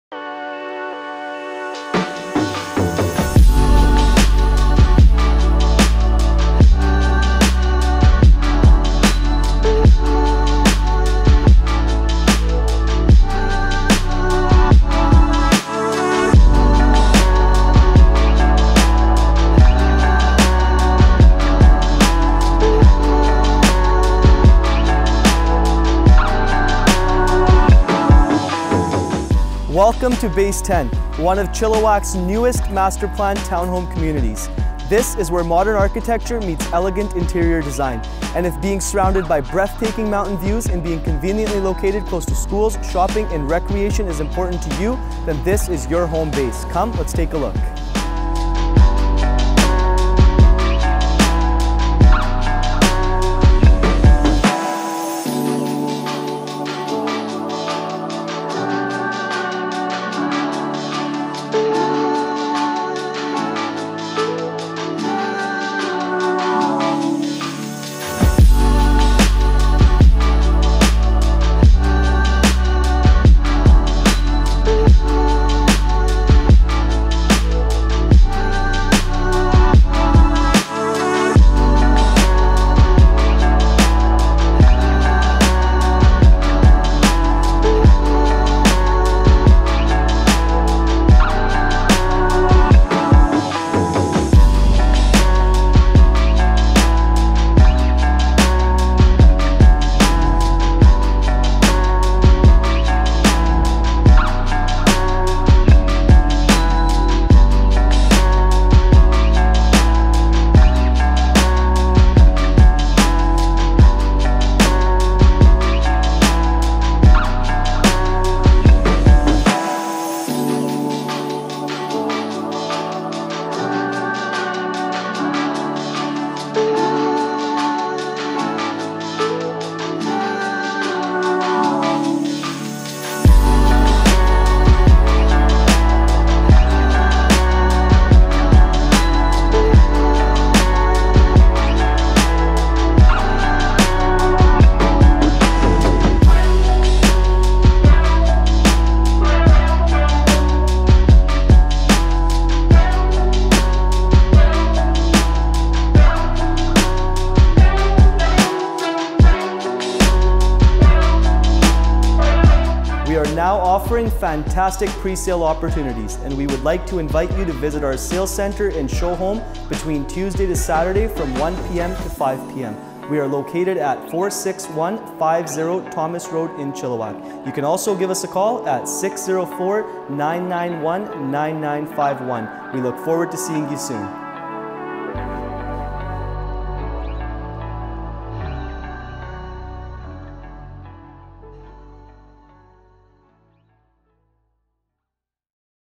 Base 10 - 46150 Thomas Road, Chilliwack BC
Base 10 - Chilliwack's newest master-planned townhome community. Great Pre-Sale opportunities available now! for more info.